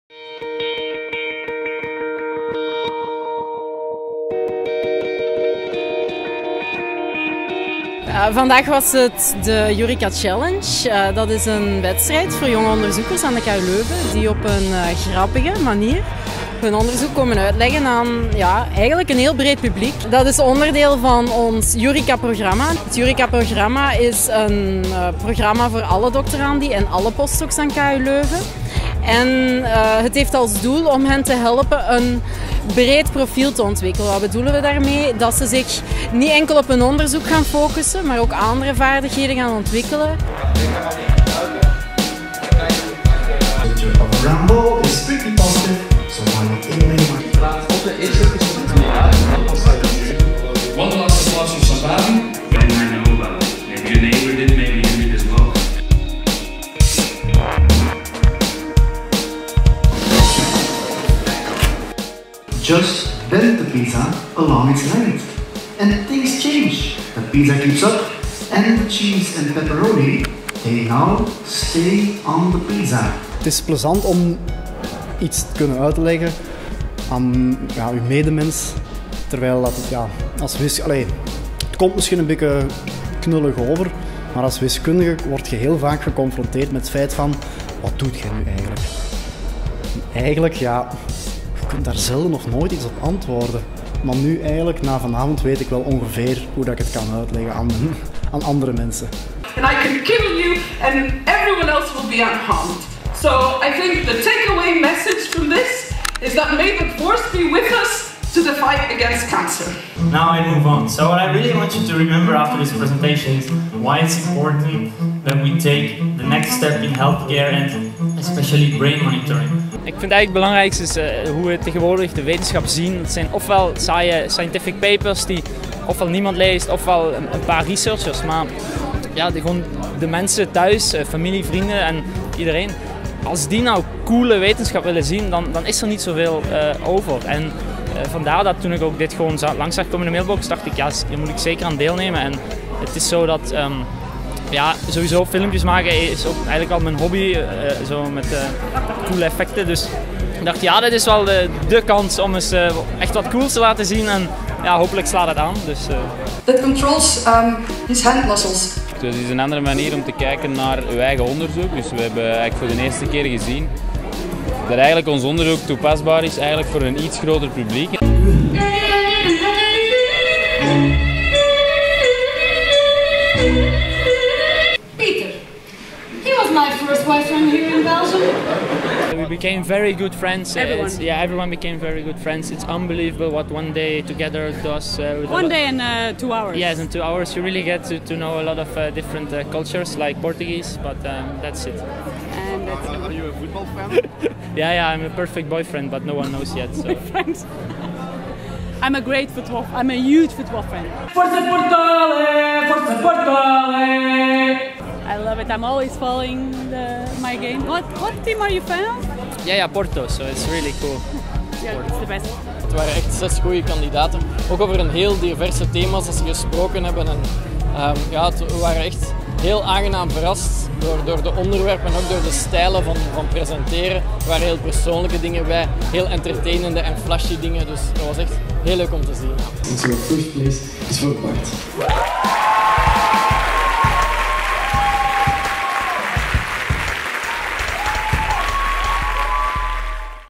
YouReCa Challenge 2013
What happens when we throw a handful of enthusiastic young researchers in front of an audience and let them compete for applause? KU Leuven challenged six (teams of) researchers to enter the stage and present a scientific problem or fact in an unconventional, funny and entertaining atmosphere. During short live performances they needed to win the hearts of the crowd and a professional jury - headed by rector Rik Torfs! Bart Dioos is the winner of the YouReCa Challenge 2013. He won over the jury and the audience with his 'Eating pizza like a pro'